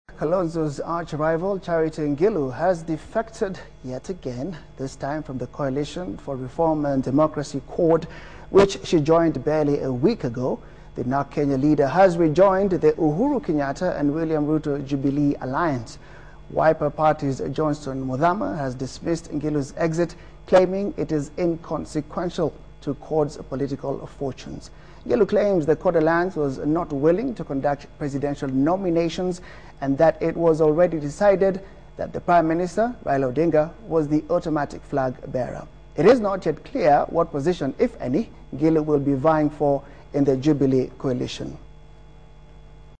News : Ngilu defects to jubilee coalition
Elsewhere, Kalonzo's arch rival Charity Ngilu has defected yet again -- this time from the coalition for reform and democracy, cord -- which she joined barely a week ago.